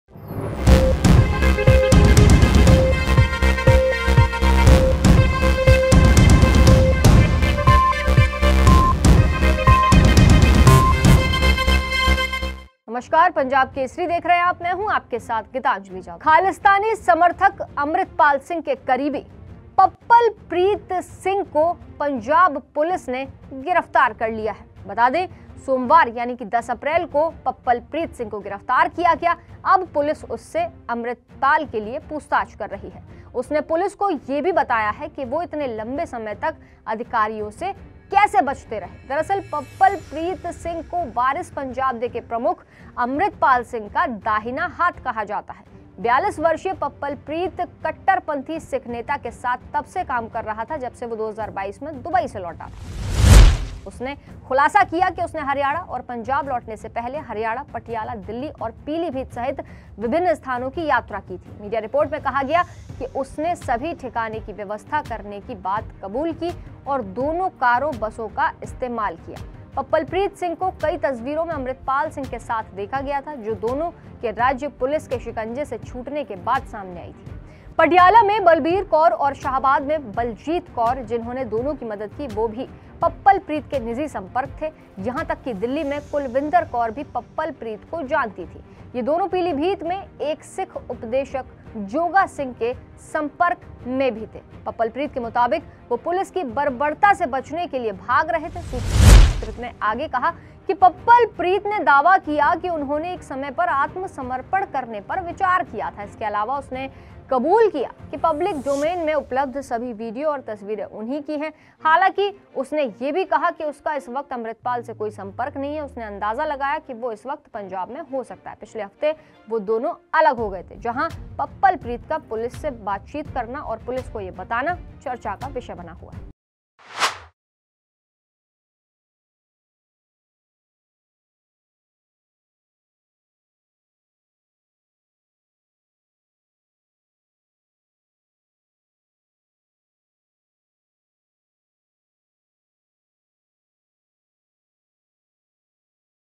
अमृतपाल के करीबी दोस्त ने गिरफ़्तारी के बाद खोले राज़, पुलिस से कैसे बच रहा है? कहां है अमृतपाल? जानिए
Pappalpreet Singh, a close aide of Khalistani supporter Amritpal Singh, was arrested by the Punjab Police on Monday (April 10). Now the police is questioning him about Amritpal. He has also told the police how he managed to escape from the authorities for so long.